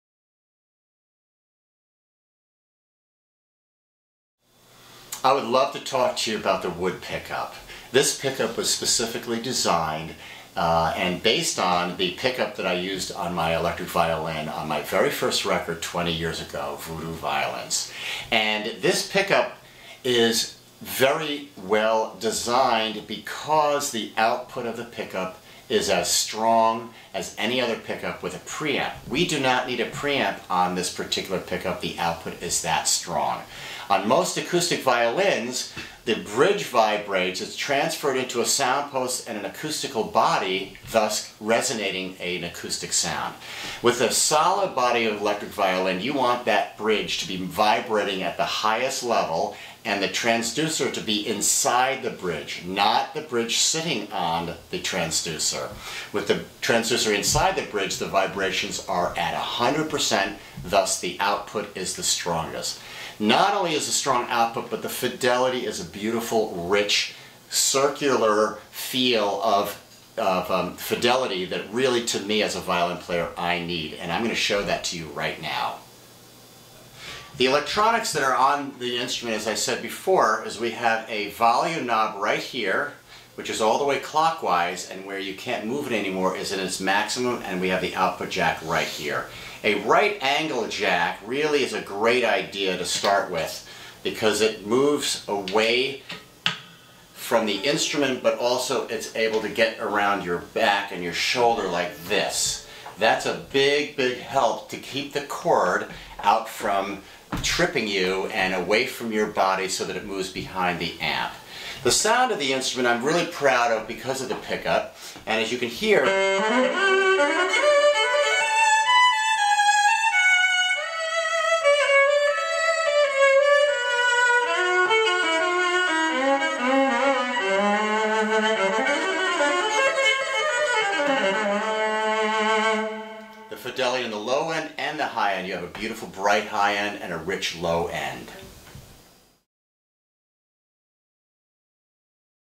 Mark Wood - Wood Violins Bridge Pickup
- Mark Wood, world-renowned electric violinist and inventor of the Viper electric violin, talks about the revolutionary designs and concepts of Wood Violins.